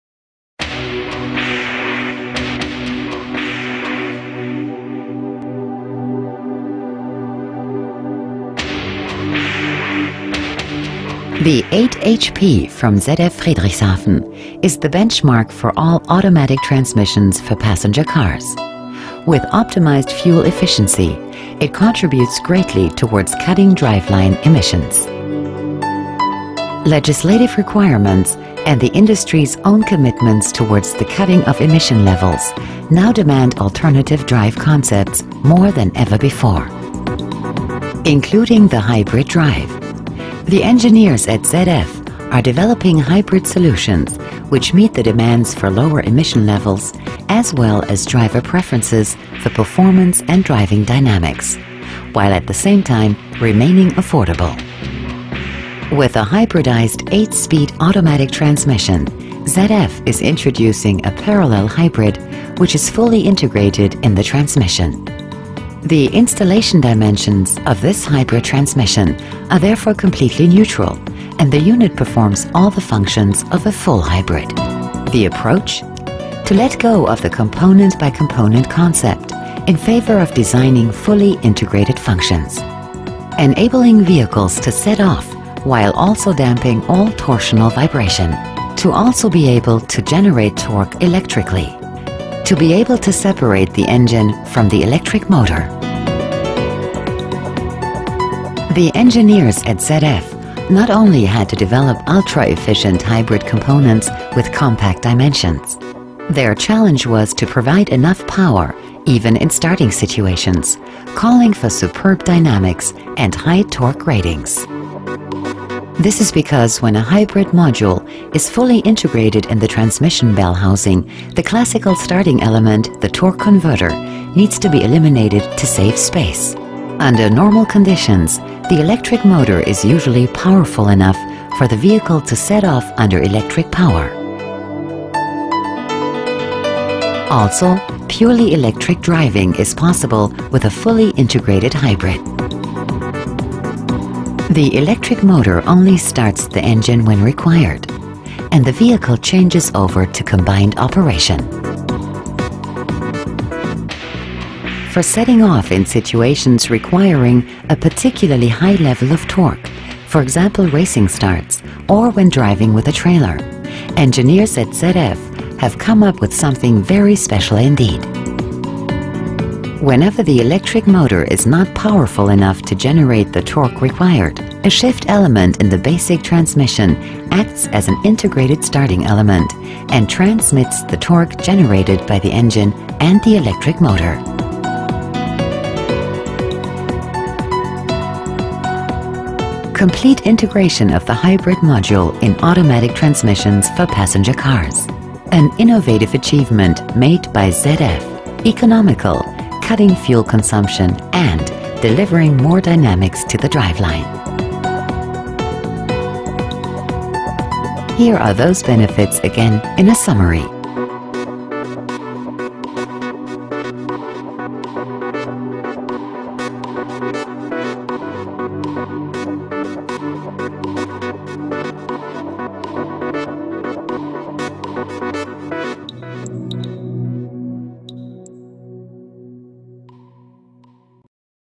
Cutia de viteze automată hibridă 8HP de la ZF
Modul de funcționare al cutiei de viteze automată hibridă 8HP de la ZF